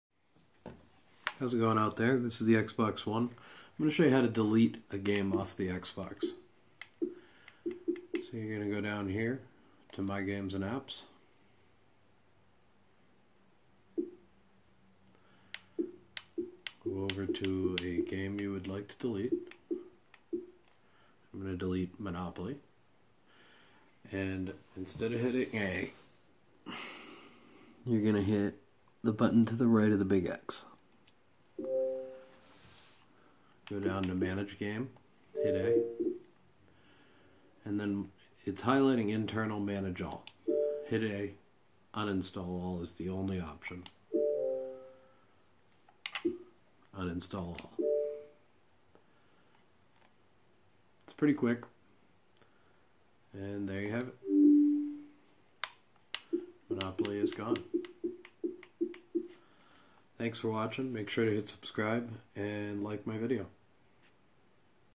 Xbox One How to delete a Game or App
How to delete a game or application off the Xbox One with the latest software.